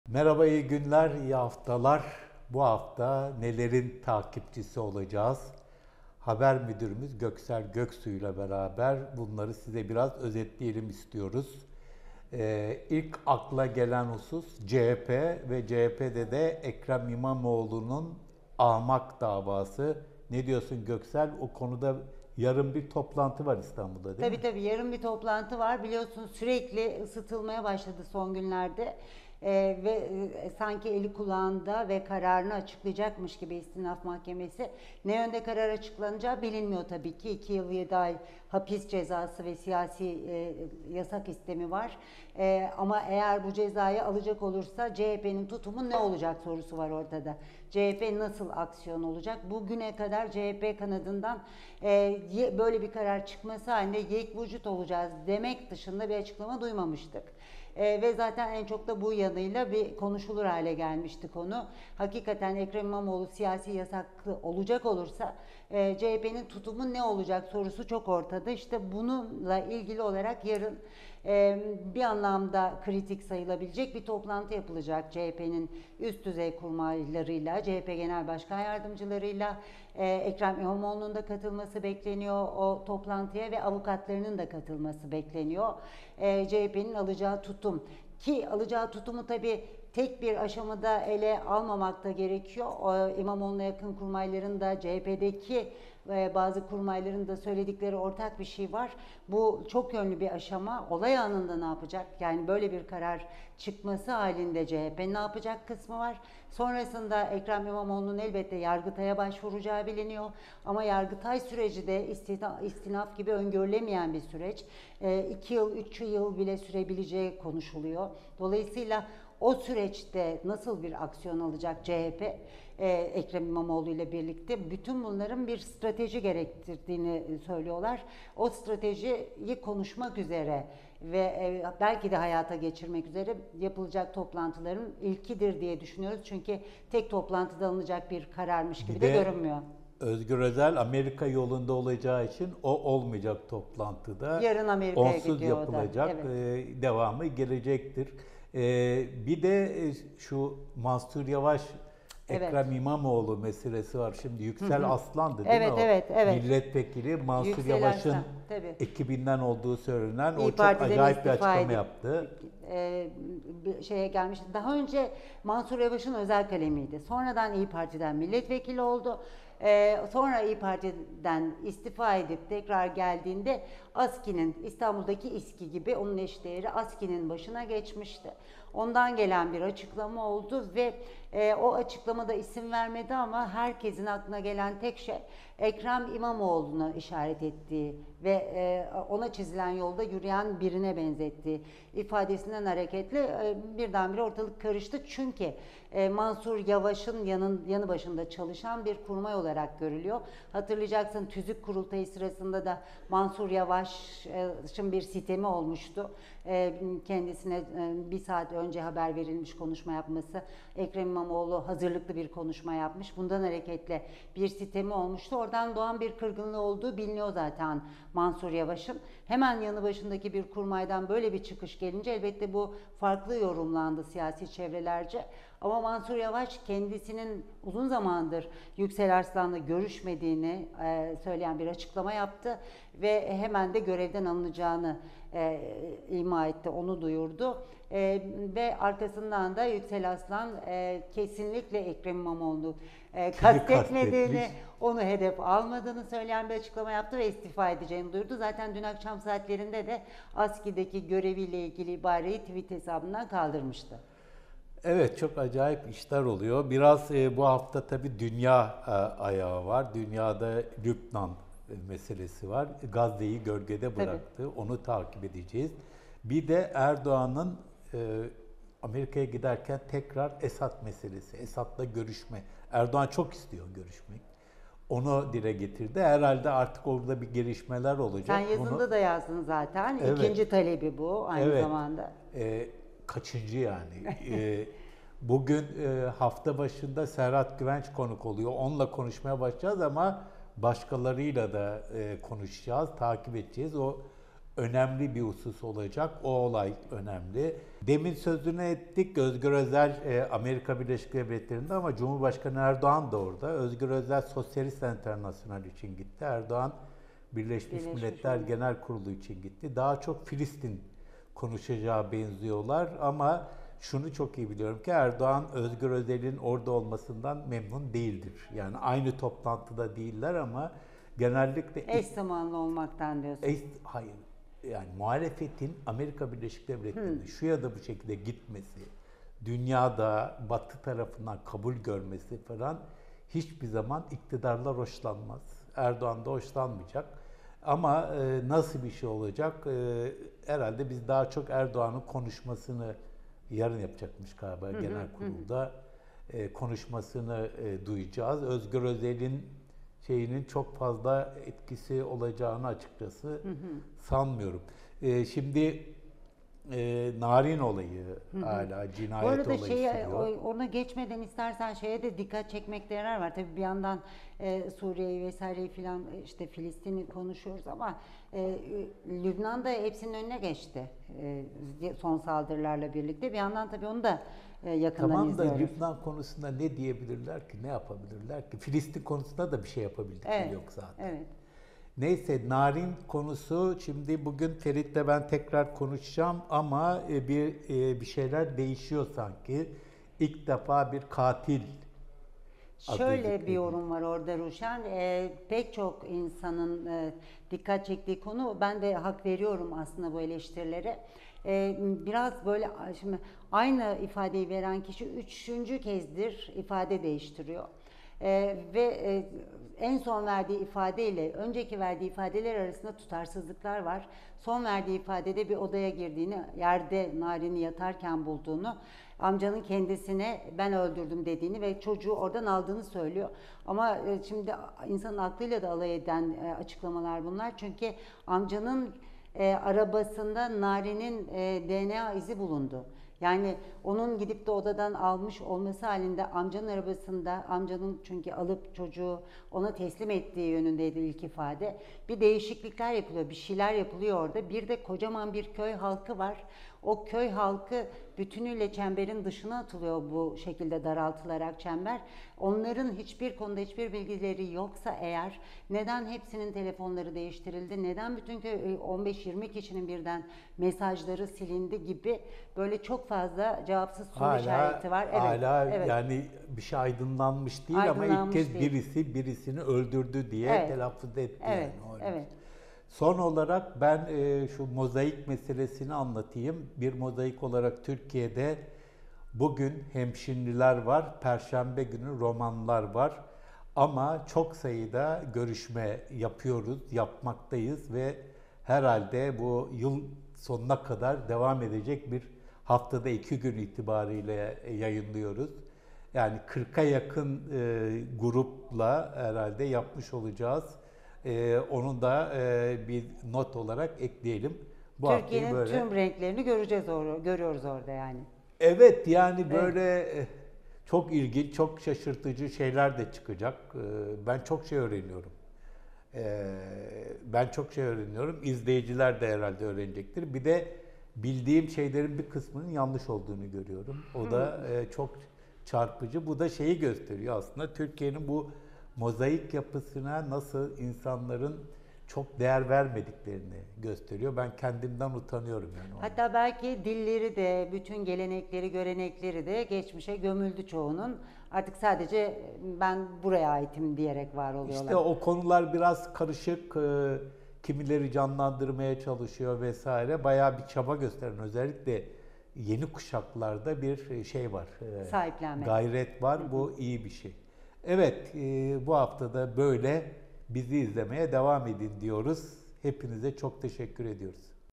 ⏰ BU HAFTA BİZİ NE BEKLİYOR? Ruşen Çakır ve Göksel Göksu sizin için değerlendiriyor!
Üyelerimize özel gündem değerlendirmemiz yayında! 🥳

📺 İyi seyirler!